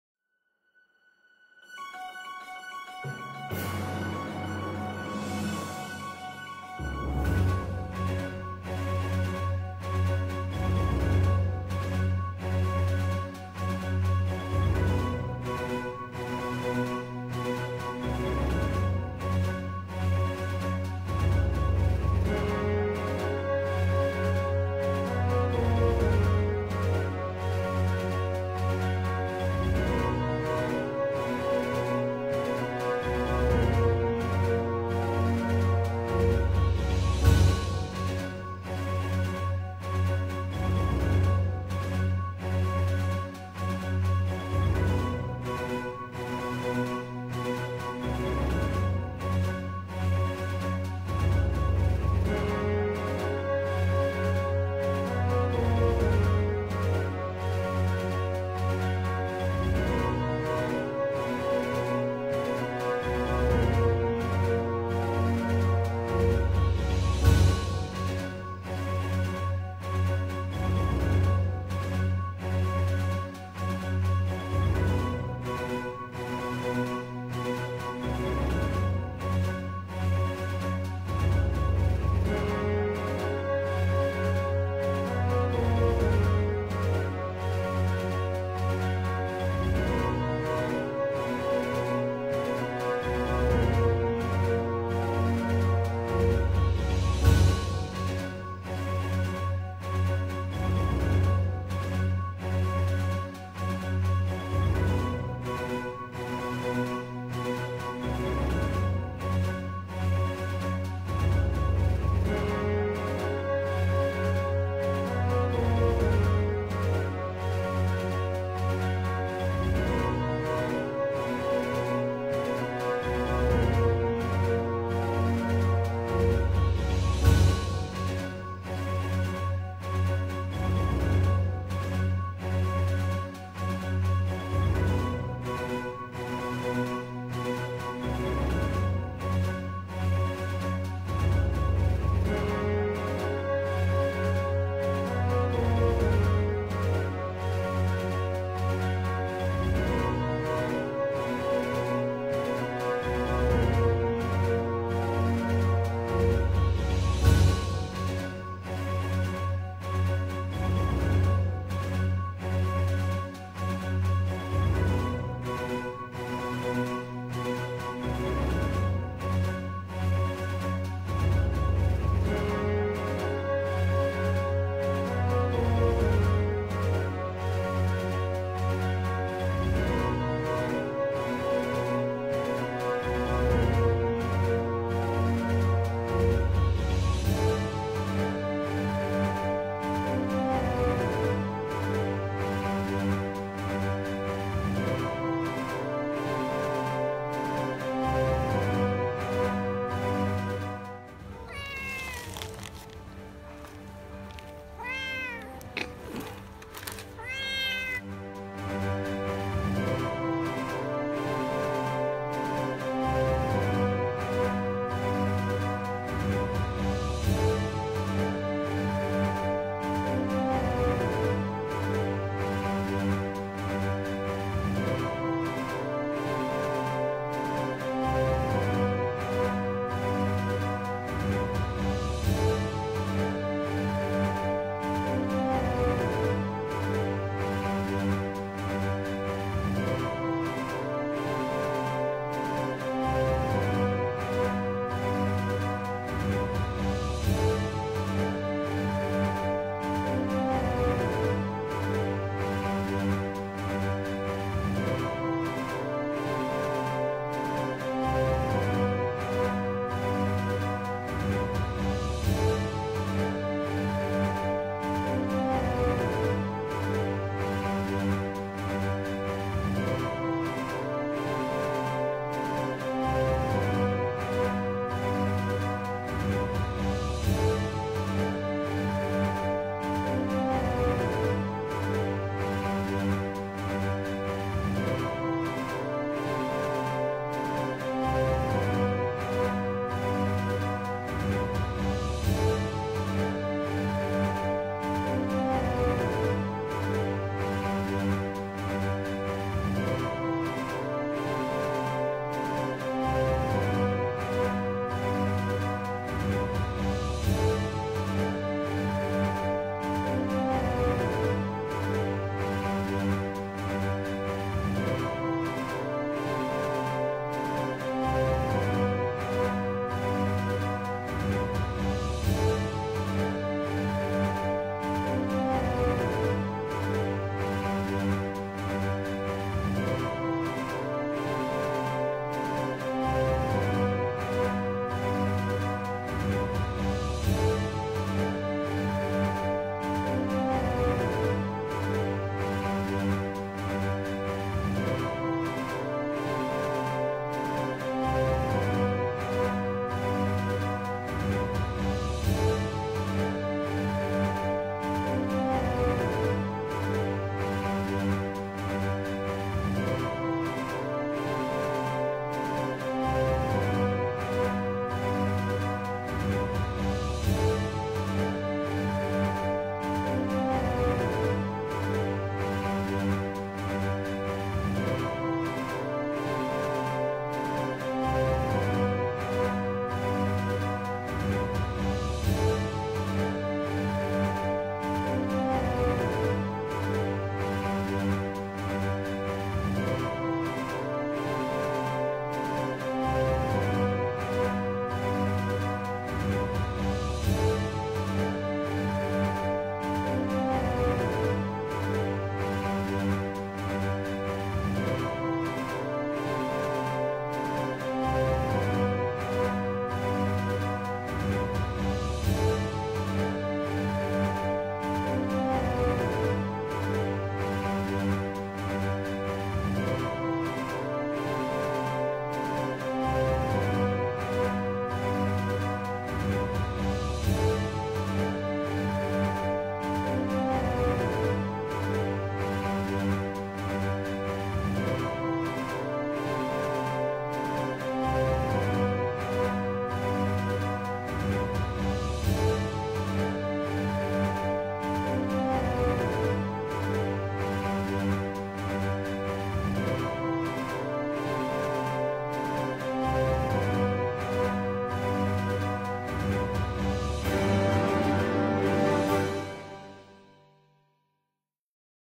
Tragic death of an injured kitten.
The injured kitten was picked up from the side of the road and could not be saved after many attempts.